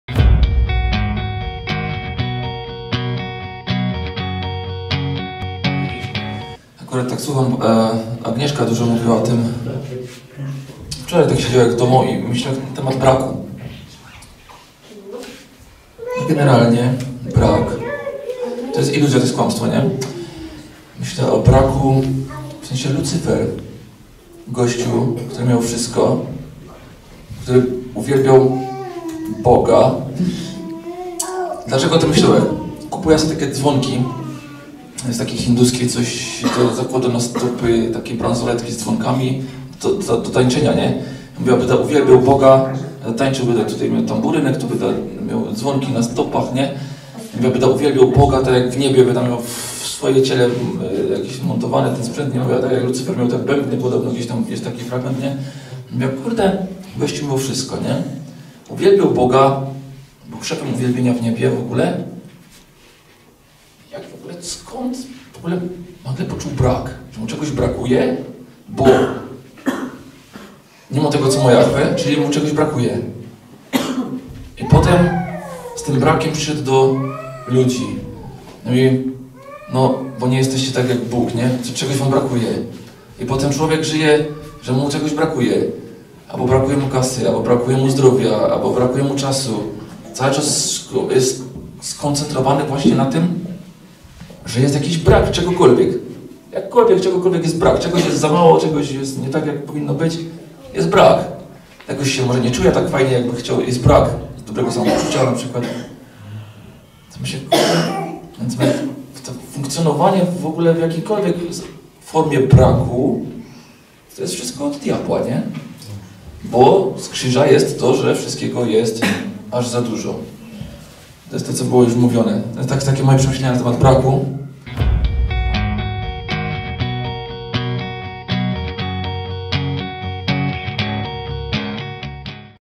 Wszelka forma braku pochodzi od diabła - Dawid Mencel (9.10.2022).
Kościół Chrześcijański "Królestwo Niebieskie" w Gliwicach.

Zajrzyj również na stronę Fundacji Królestwo!